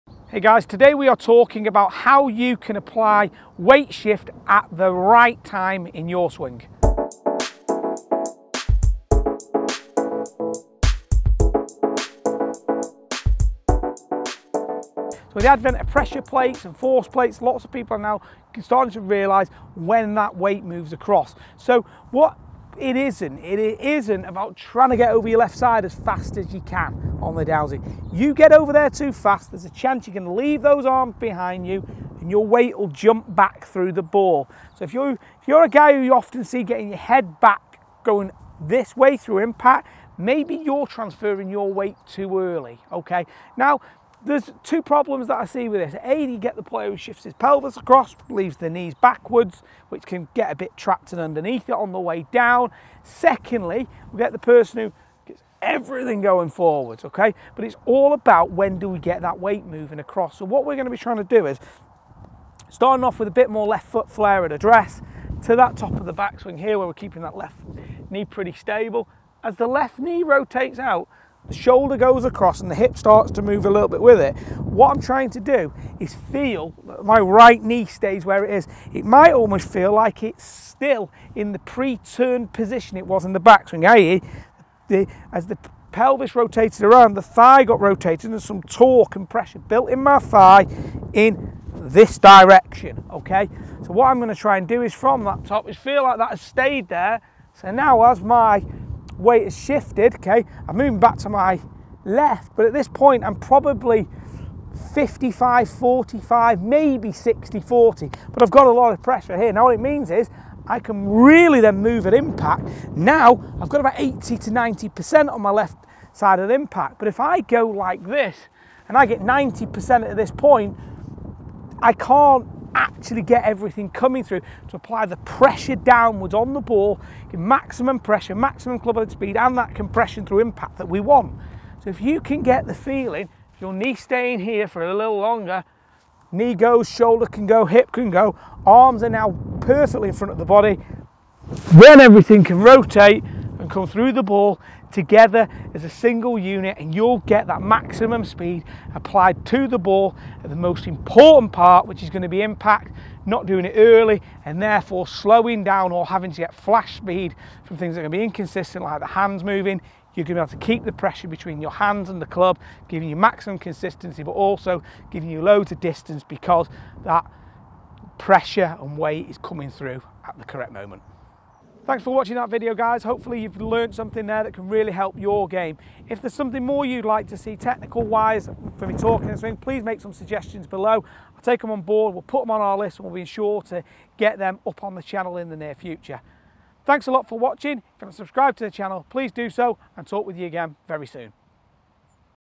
CORRECT DOWNSWING WEIGHT SHIFT
Timing your weight shift properly allows you to deliver the club in the best way to get the maximum compression at impact. In this video Dan talks about how to shift your weight in the downswing.